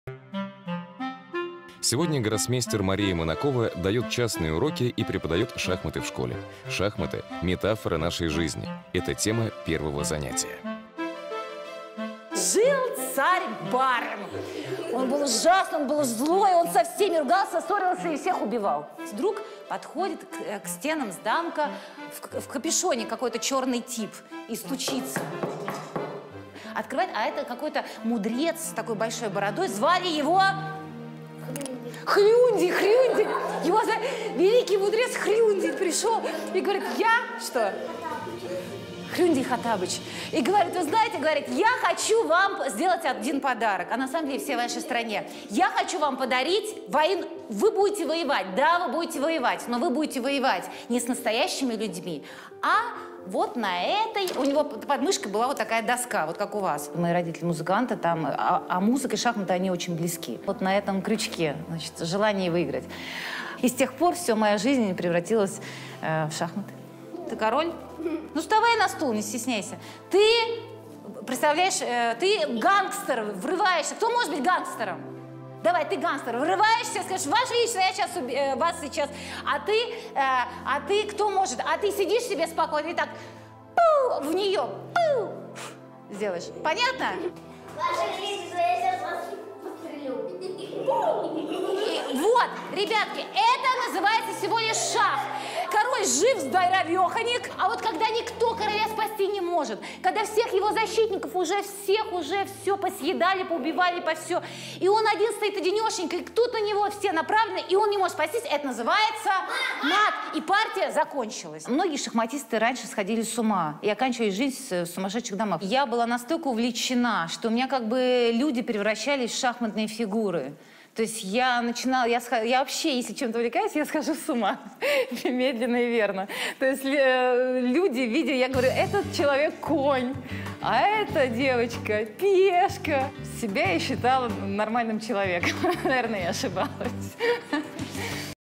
Урок шахмат Марии Манаковой
Эпизод документального фильма о гроссмейстере Марии Манаковой - занятие шахматами в школе.